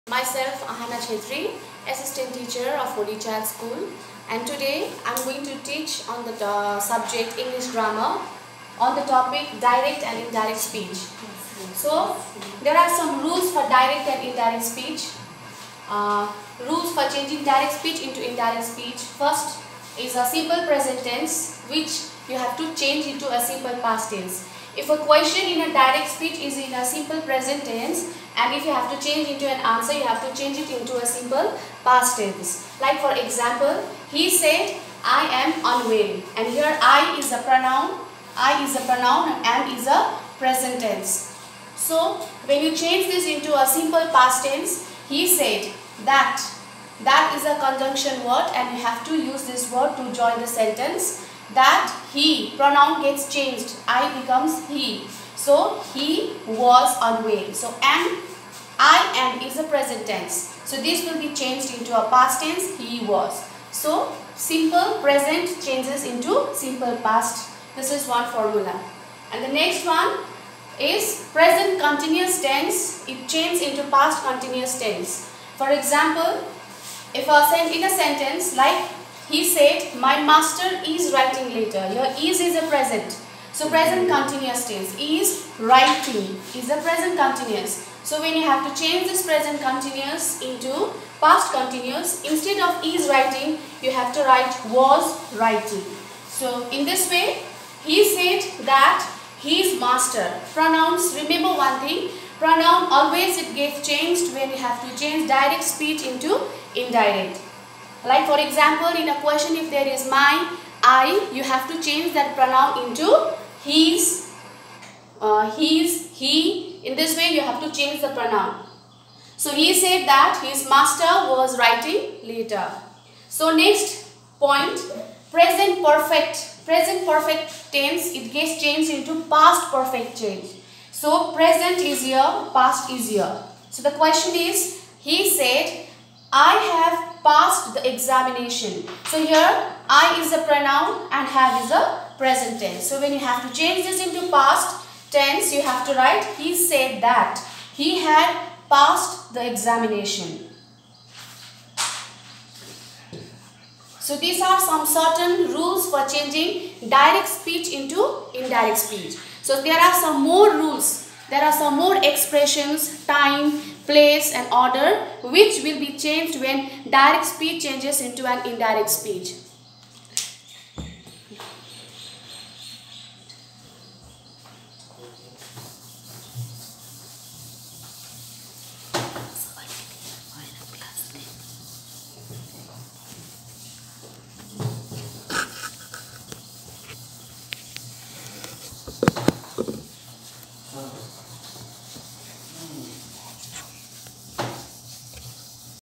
Online Education Episode 2 Part-1, English Grammar for Classes VII to XII.
Topic: Direct and Indirect Speech, Subject Teacher: Mrs. Aahana Chhetri. HCS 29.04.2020